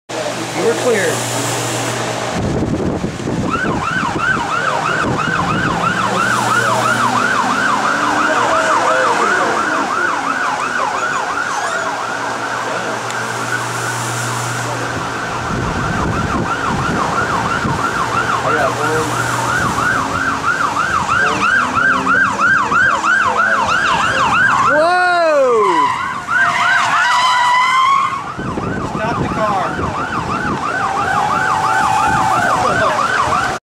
Law Enforcement Recruit Spins Car
During "cat and mouse" a police recruit loses traction in the rear tires and spins the car.